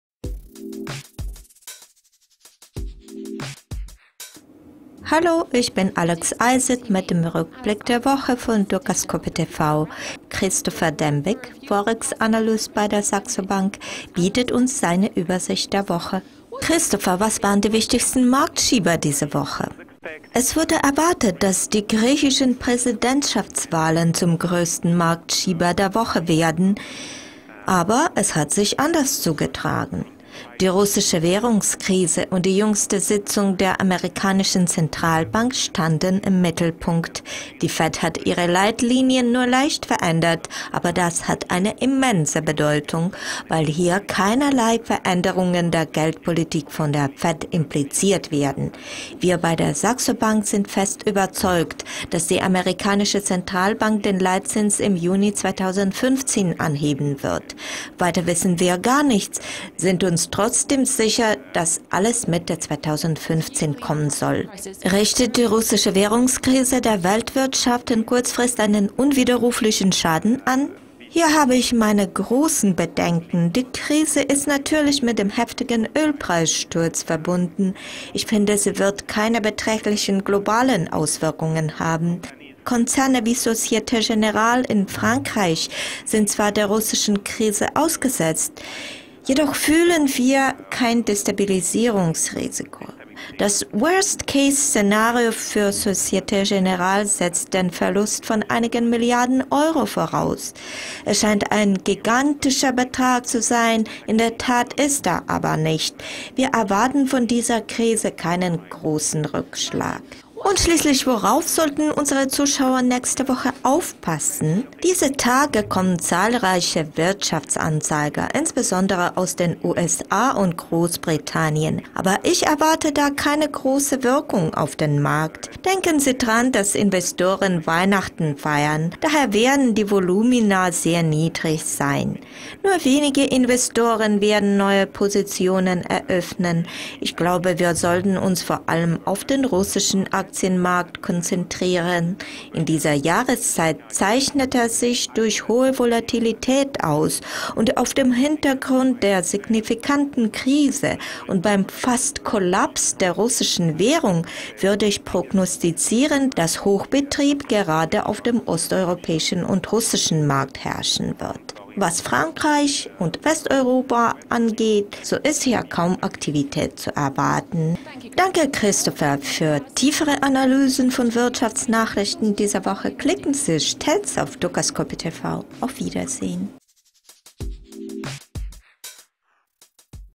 Russland in Mittelpunkt
Die griechischen Wahlen verblassen auf dem Hintergrund der russischen Sorgen. Christopher Dembik, Saxo Bank